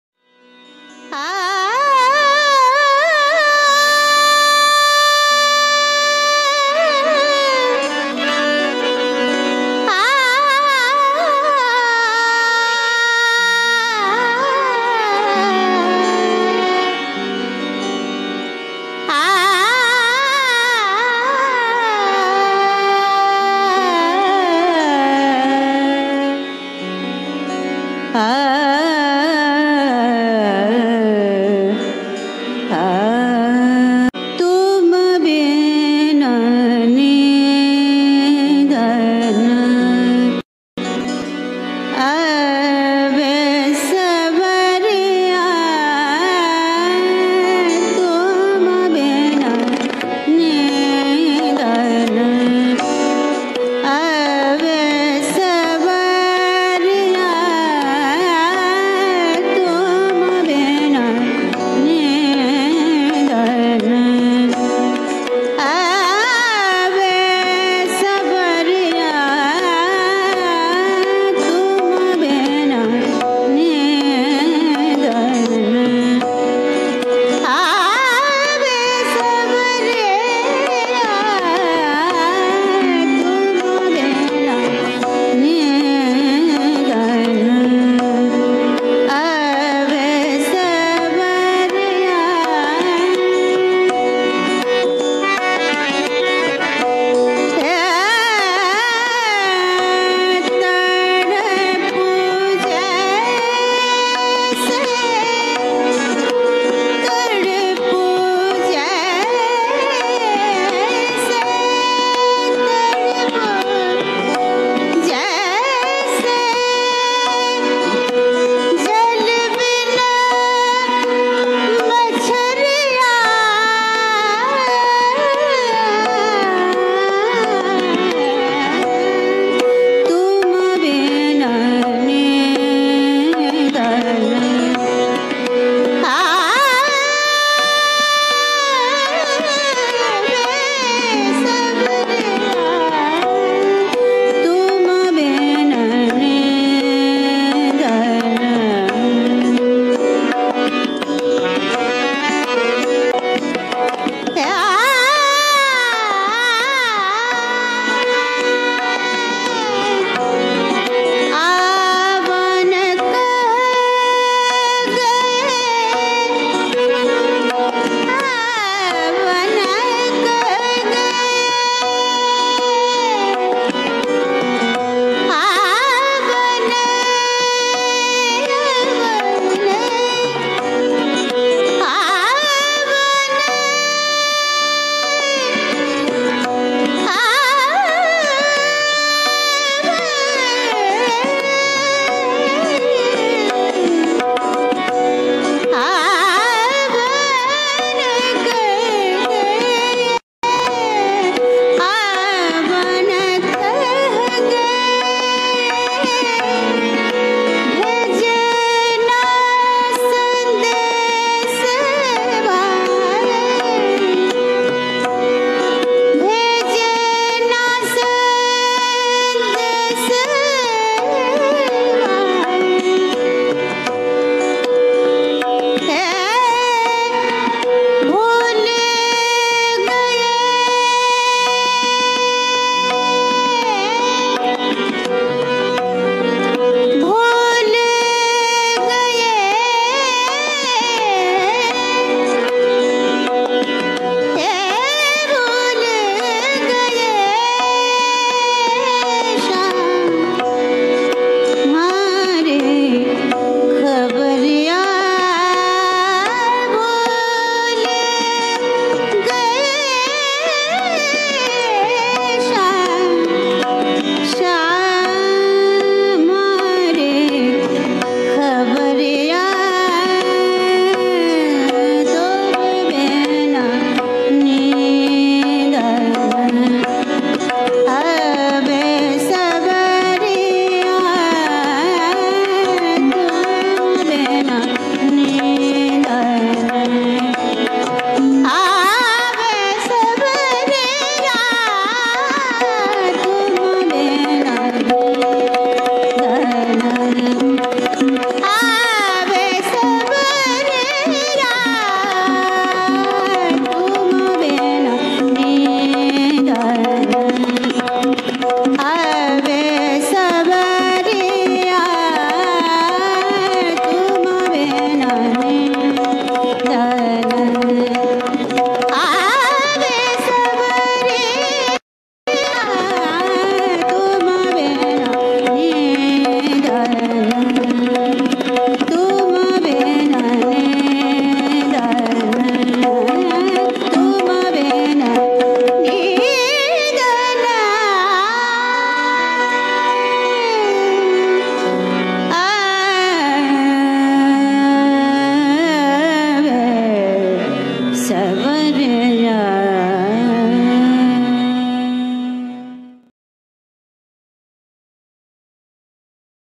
Thumri - Koushambi Mukhopadhyay
World Music Day (2021) as observed by Music Department of Bankura Zilla Saradamani Mahila Mahavidyapith.